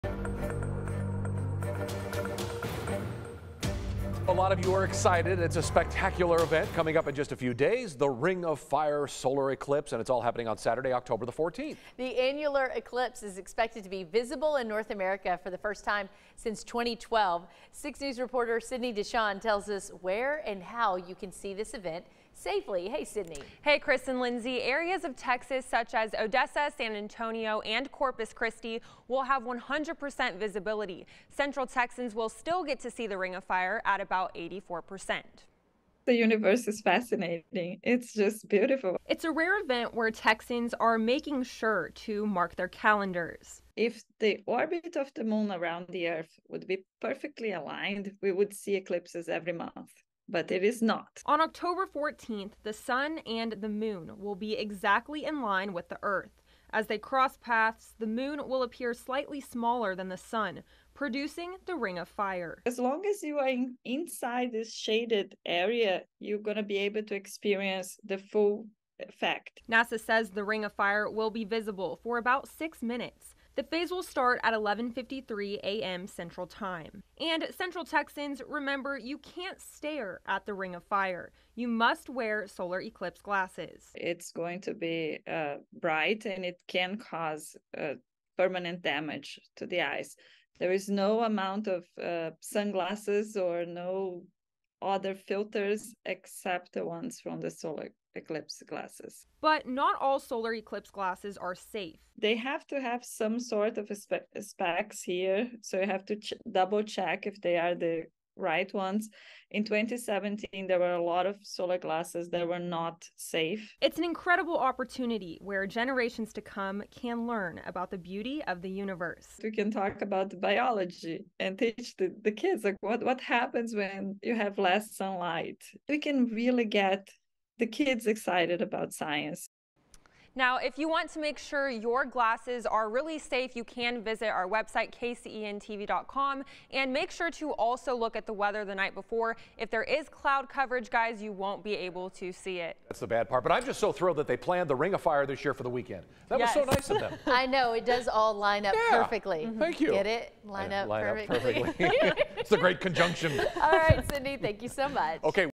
'Don't forget your eclipse glasses' | Preparing for the 'Ring of Fire' Solar Eclipse, what you need
The annular solar eclipse is expected to be visible in North America for the first time since 2012 on Oct. 14.